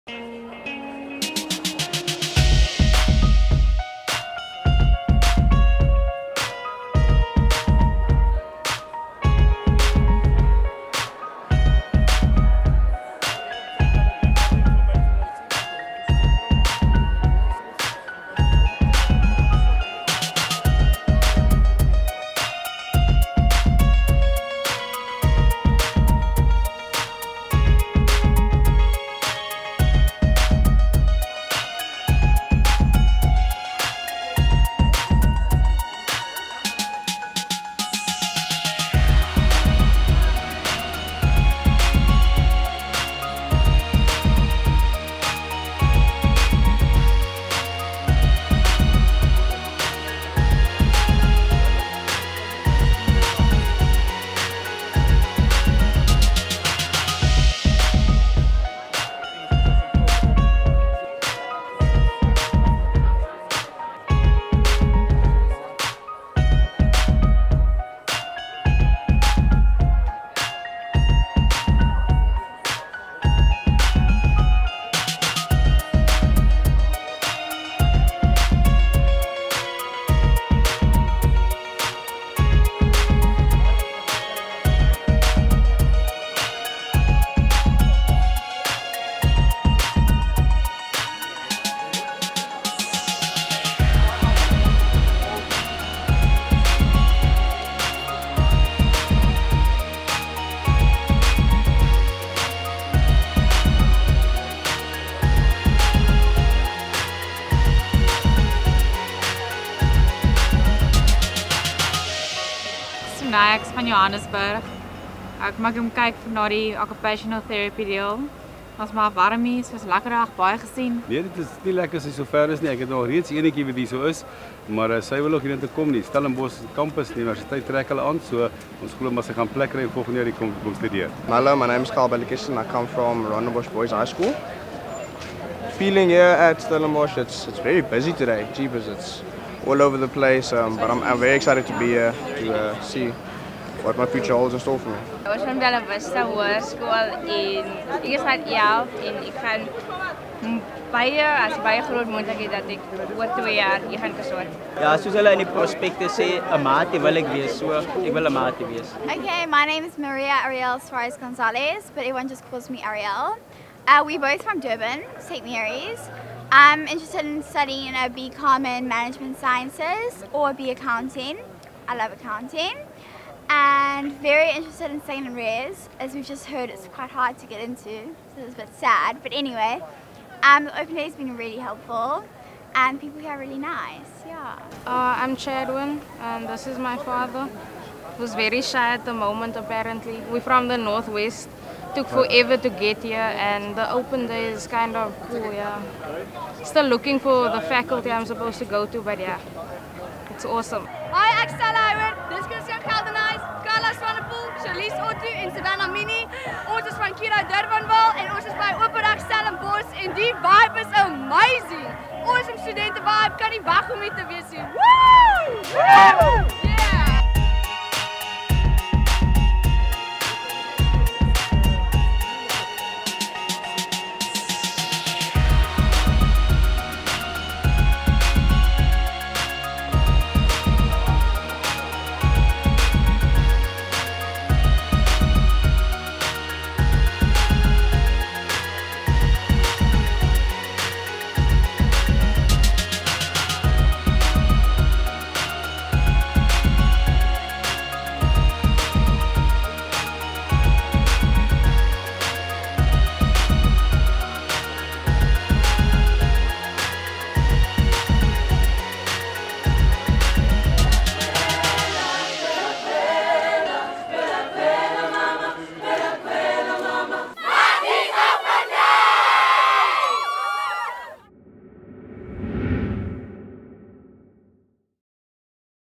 Maties Open Day 2014
Here are some highlights of the Maties 2014 Open Day.